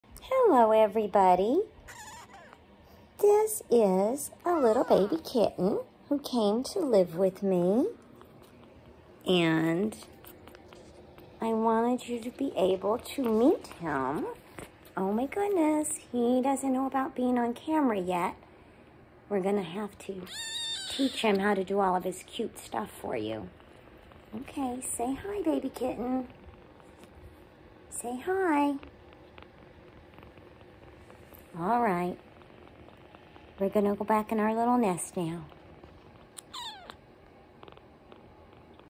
Tiny baby kitten makes adorable squee sounds!
This is the orphaned baby kitten who was given to me yesterday. He’s a good eater and I hope I can find him a good home.🐱